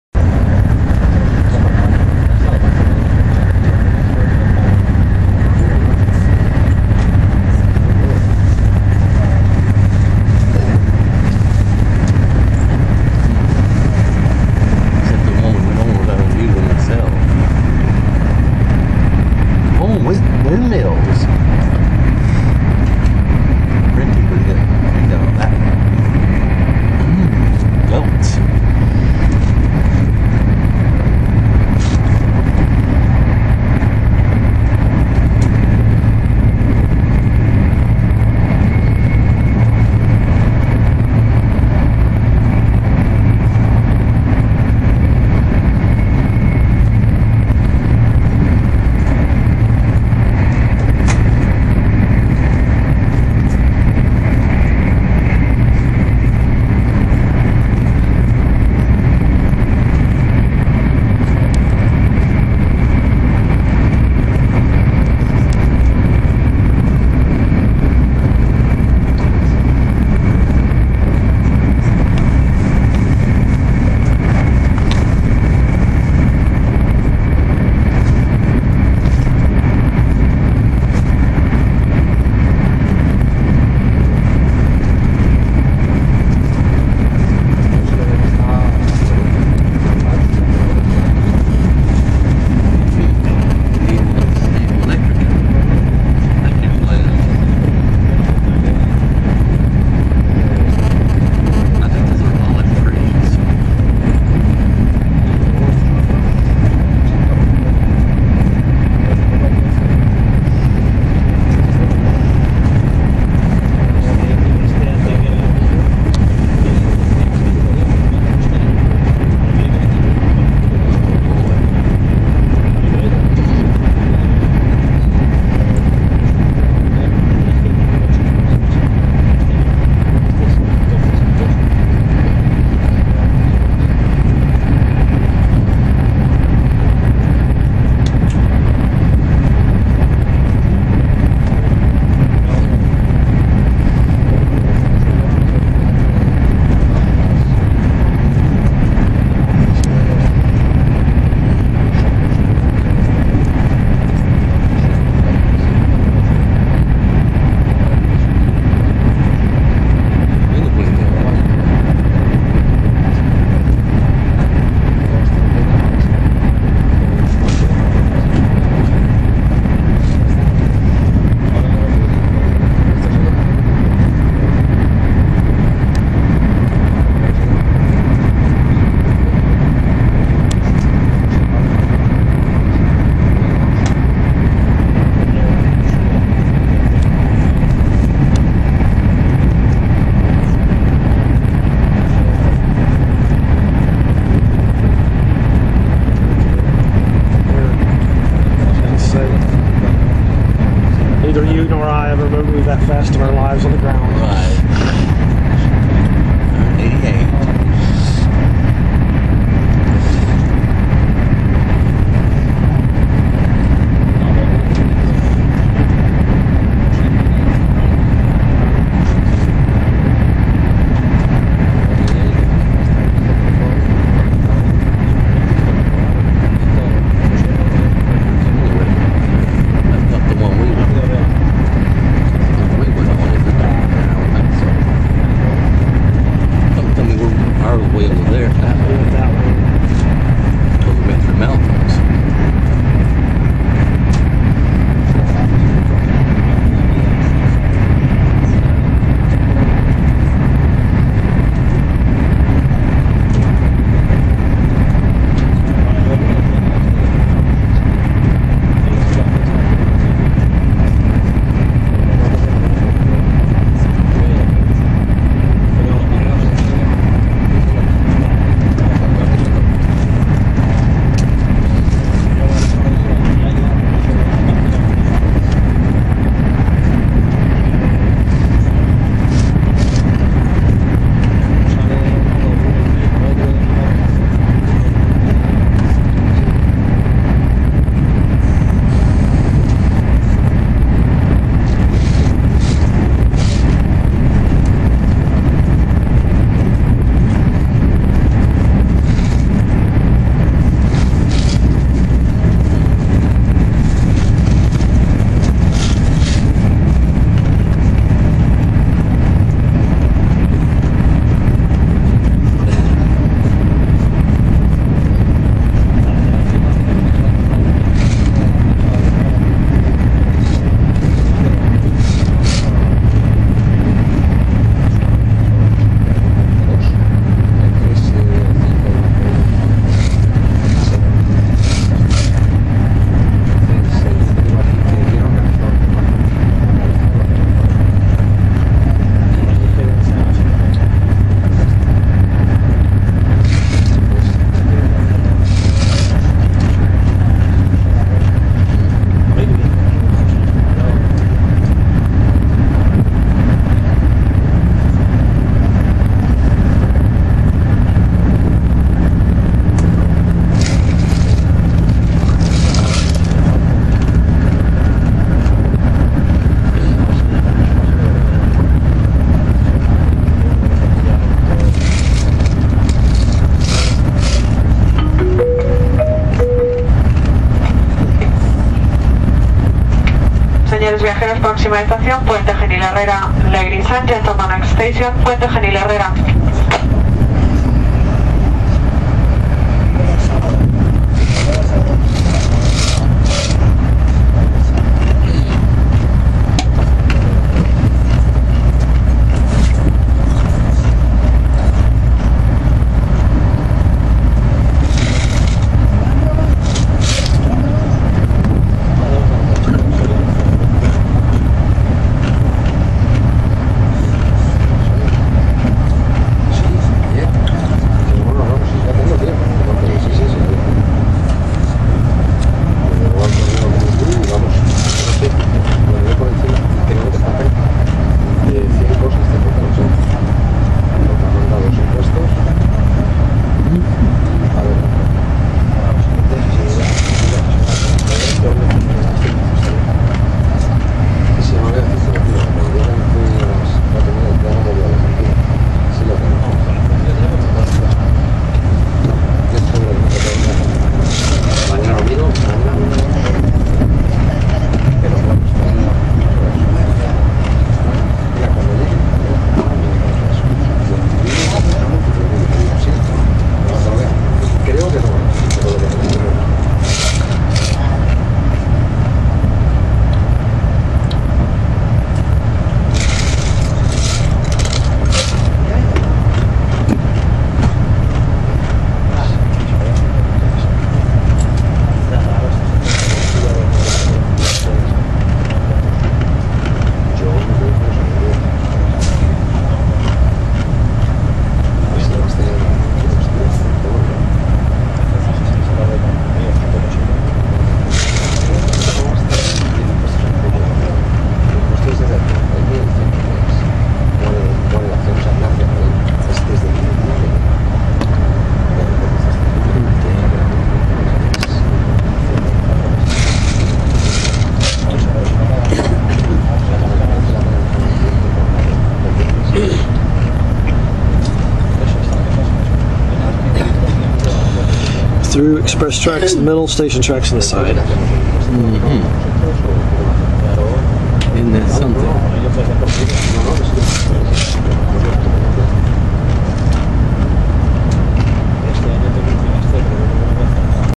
On the AVE high-speed train at 180 mph
Traveling on the AVE from Malaga to Cordoba.  MPH checked on my iPhone 4s. This is north of Antequera the afternoon of October 23, 2012.